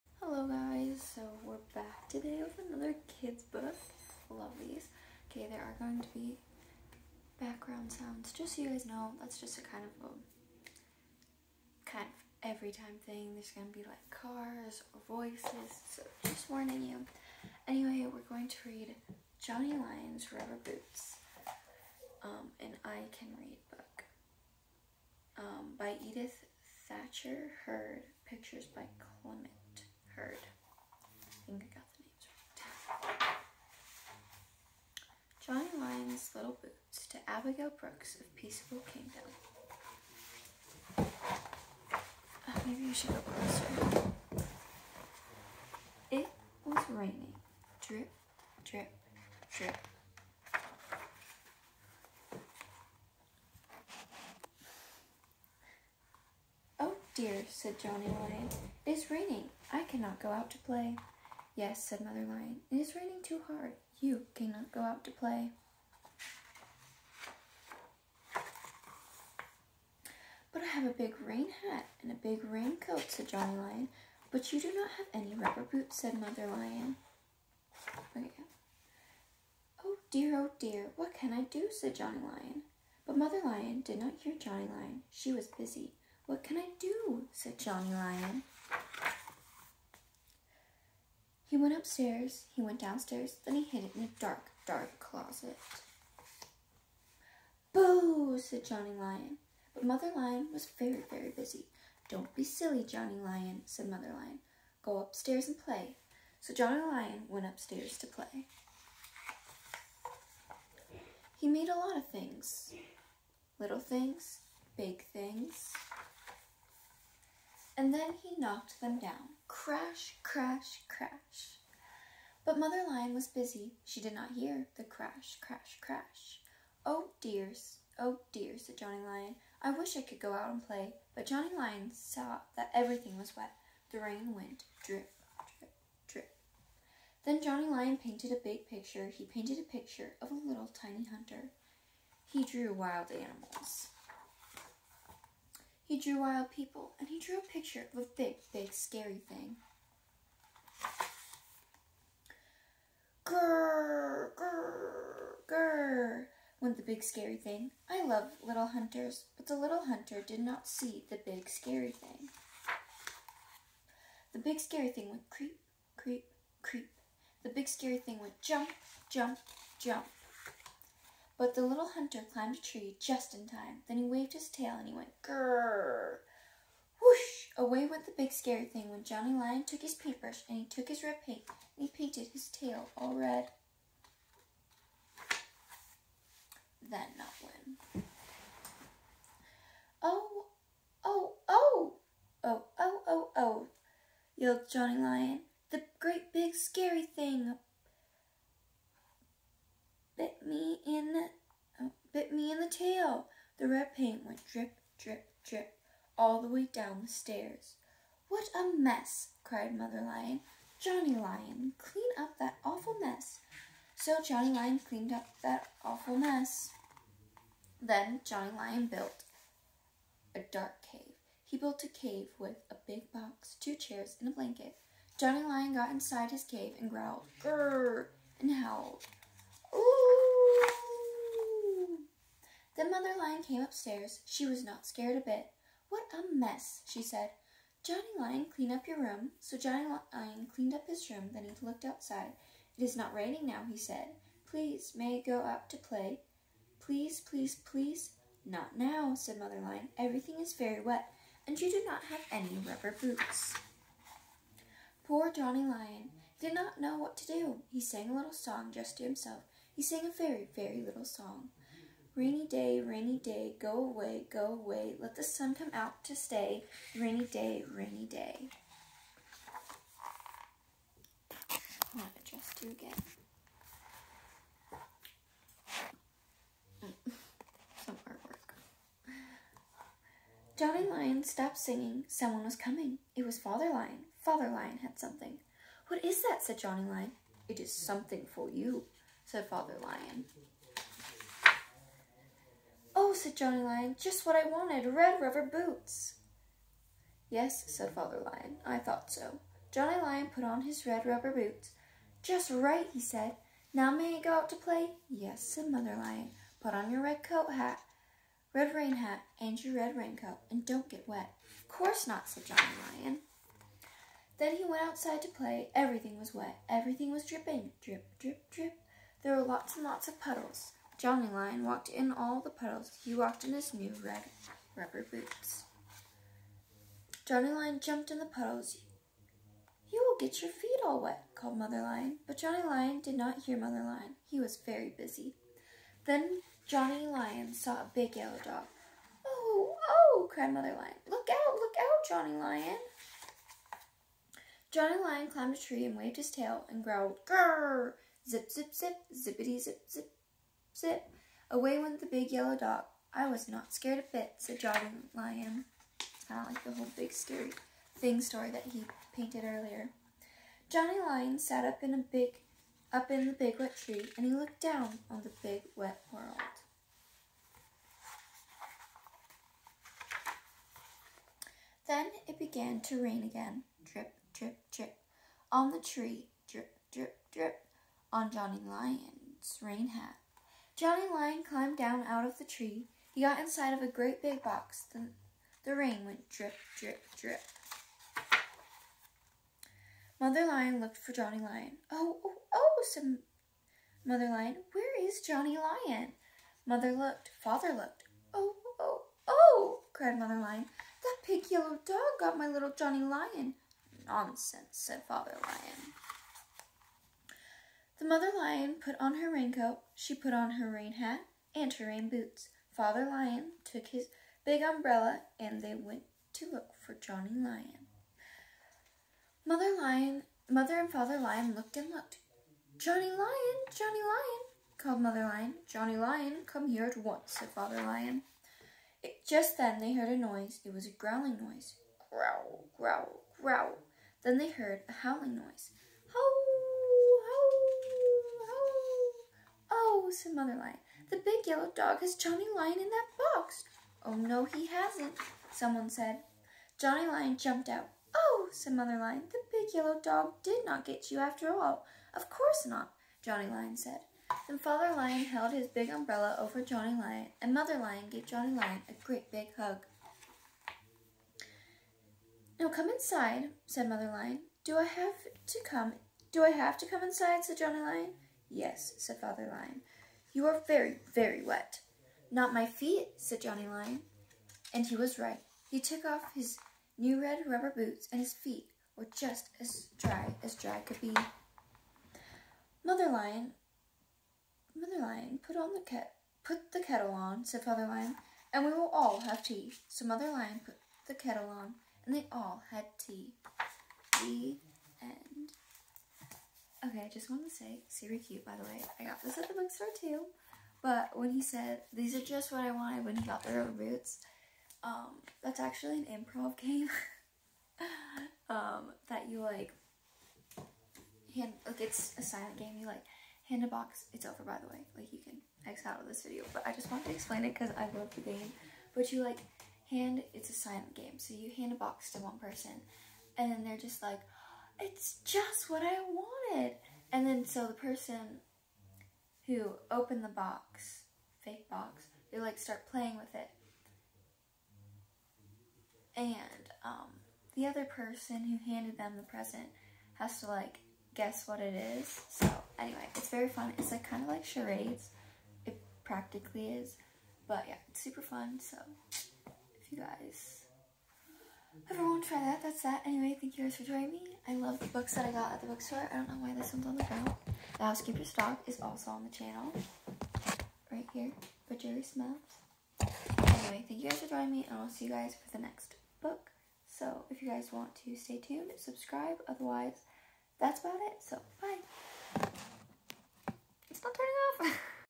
Johnny Lion's Rubber Boots by Edith Thacher Hurd pictures by Clement Hurd
This is an "I can read book" by Edith Thacher Hurd pictures by Clement Hurd.  I love this! It is so cute! I picked it up at the same bookstore as the last two I have read.  love kid books!
I hope you guys enjoyed or enjoy this book! 😊❤️